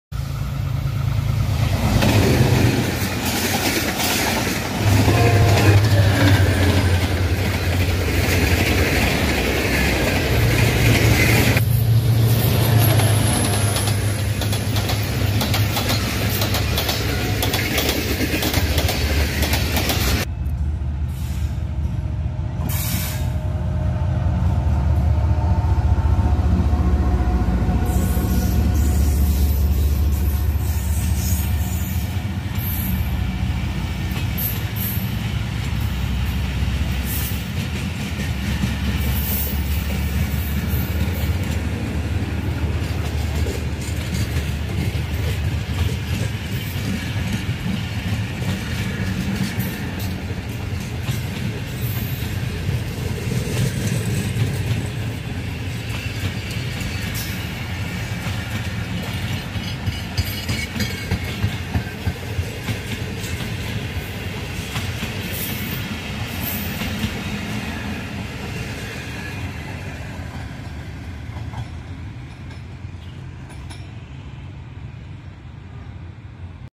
Long Route Train Tezgam Express Departing Okara Railway Station Credits
Long Route Train Tezgam Departure from Okara Railway Station of Pakistan Railways. Okara Railway Station is situated at a distance of 9 Kms from Kissan Railway Station on Main Line 1.